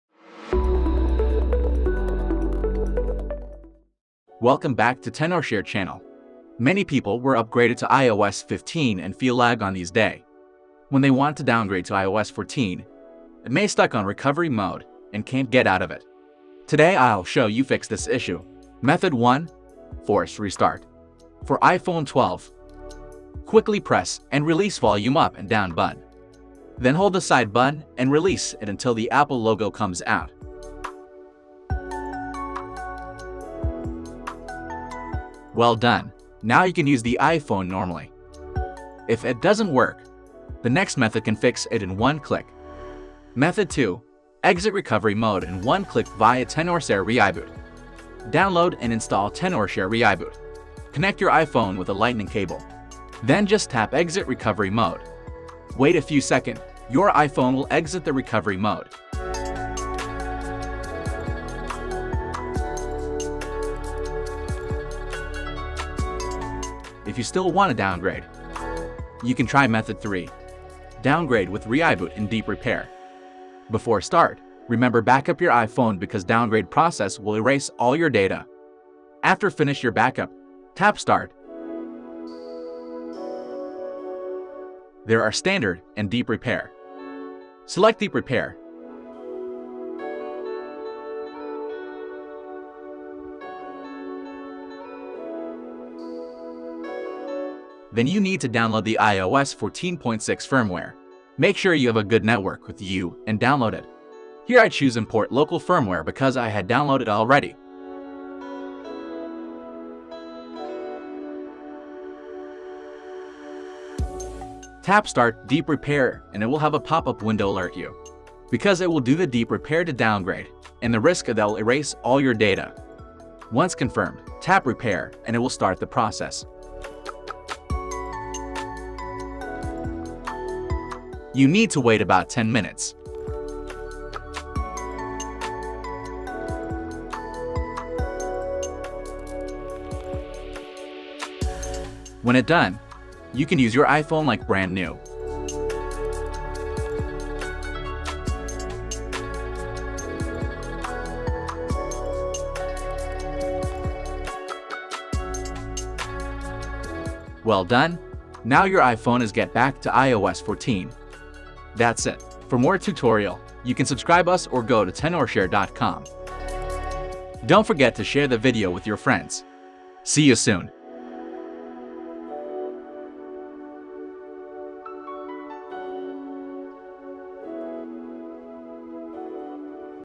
3 Methods Fix iOS15 Downgrade Stuck in Recovery Mode
Many people were upgraded to iOS 15 and feel lag on these day. When they want to downgrade to ios 14, it may stuck in recovery mode and can’t get out of it. Today I will show you fix this issue. Let's Get it!

TimeStamps
00:00 Intro and preview
00:19 Method 1 Force Restart
00:43 Method 2 Exit Recovery Mode in one click via ReiBoot
01:10 Method 3 Downgrade with Tenorshare ReiBoot